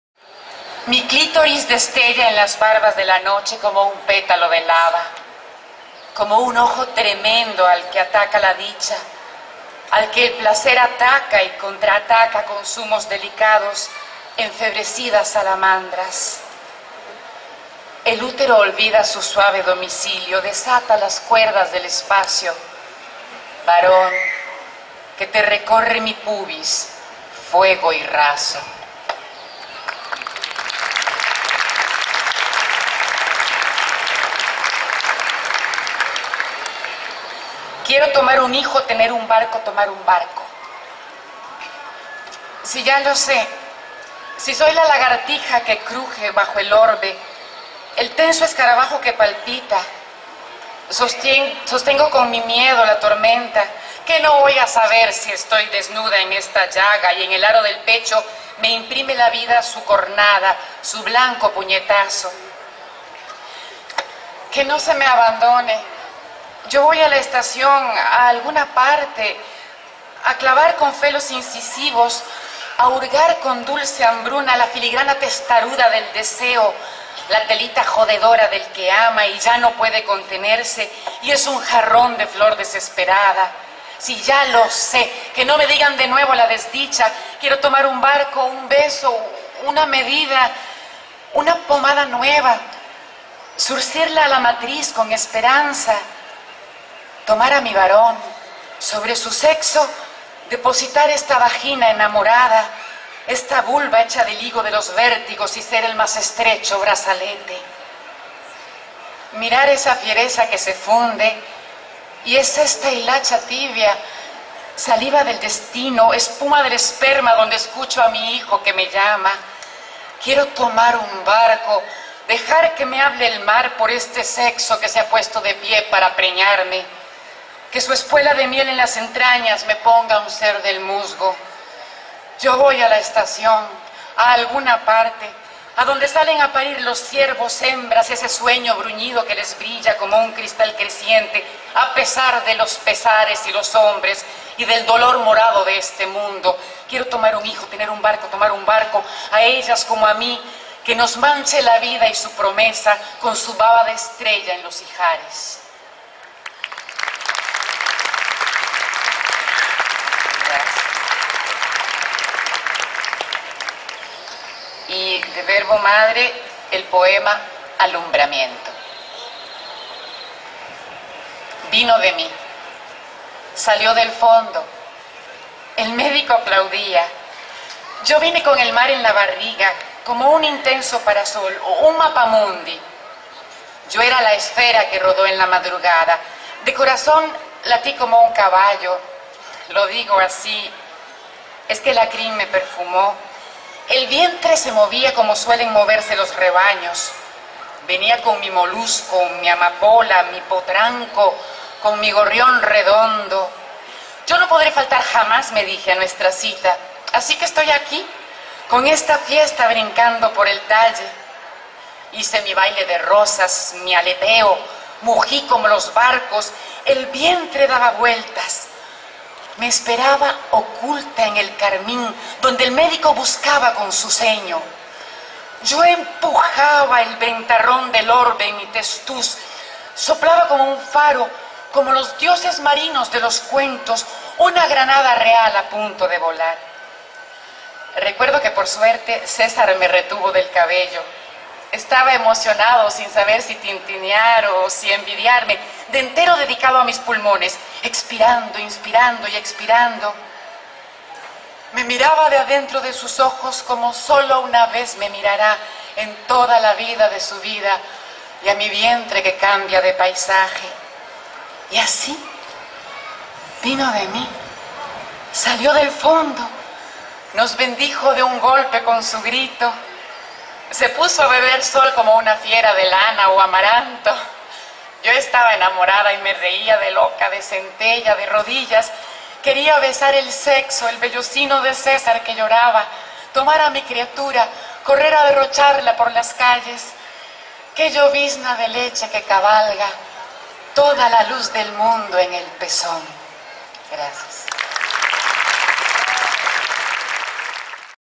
Ana Istarú. Costa Rica
Participación de la escritora en el V Festival Internacional de Poesía de Granada Nicaragua